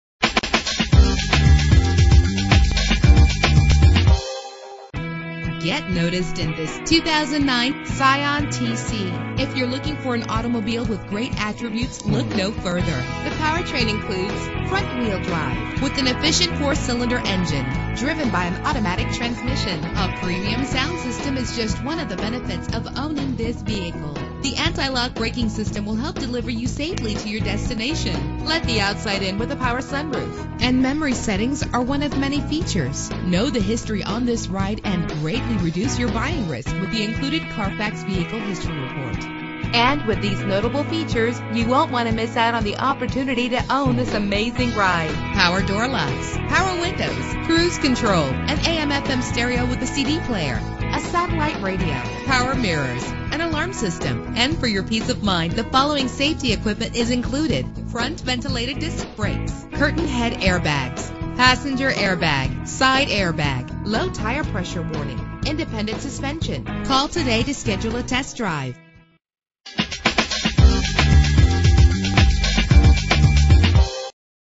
2009 Scion tC - Avenel New Jersey
Year: 2009
Make: Scion
Model: tC
Trim: Base
Engine: 2.4 liter inline 4 cylinder 16 valve
Transmission: Automatic
Color: White
Mileage: 63941
Address:
90-100 Route 1 North